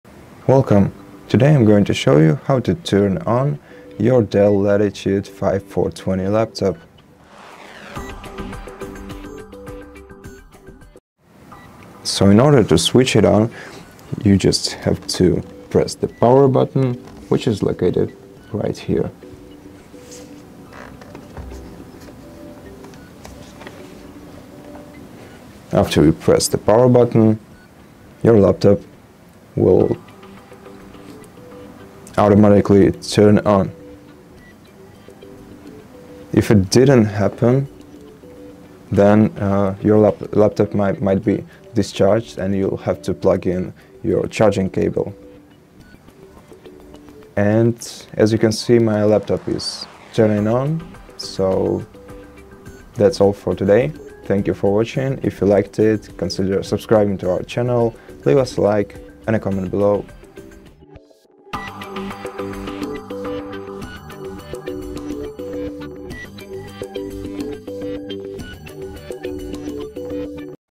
DELL Latitude 5420 – How to Switch On
If you just got a DELL Latitude 5420 and are unsure how to turn it on, this video will help you get started. We show you exactly where the power button is located on the DELL Latitude 5420 laptop and explain the simple steps to switch it on. If your laptop does not power up, we also cover what to do if the battery is discharged and how to connect the charging cable. This guide is perfect for new users or anyone having trouble powering on their DELL Latitude 5420. Learn how to start your DELL Latitude 5420 quickly and easily.

How to switch on DELL Latitude 5420 laptop?
How to find the power button on DELL Latitude 5420?
What to do if DELL Latitude 5420 does not turn on?

0:00 Introduction
0:07 Locating the power button on DELL Latitude 5420
0:19 Pressing the power button to switch on
0:32 Troubleshooting if the laptop does not turn on
0:47 Plugging in the charging cable
0:55 Laptop turning on and closing remarks